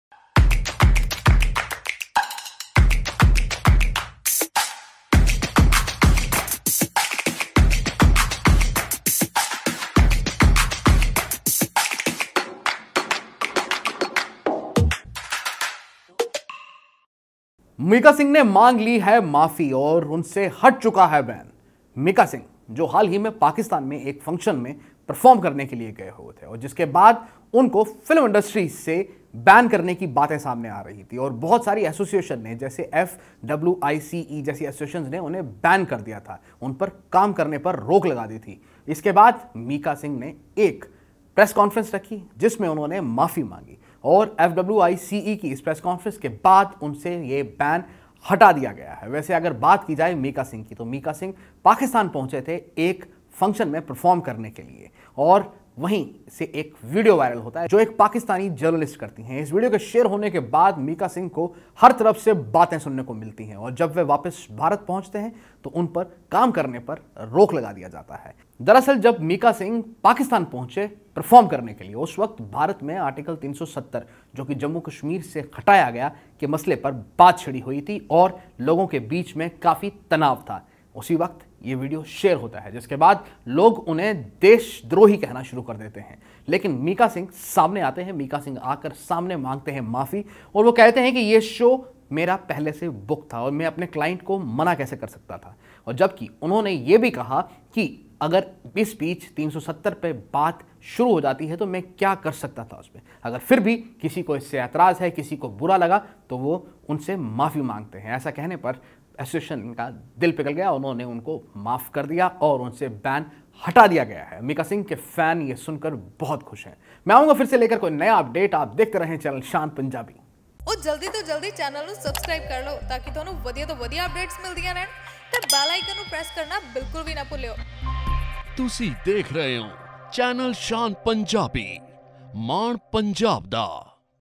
Mika Singh ने मांगी माफ़ी | उनपर लगा BAN हटा
Mika Singh has apologised to the Federation of Western India Cine Employees (FWICE) after the association banned him from working in India, following his recent live performance in Pakistan.

Mika was interacting with the media along with chief advisor of FWICE, Ashoke Pandit, and the body's president, B.N. Tiwari, on Wednesday in Mumbai. He said he had to do it owing to prior commitment, but admitted it was a mistake that he would never repeat. However, the interaction soon turned into a heated exchange between the singer and the media.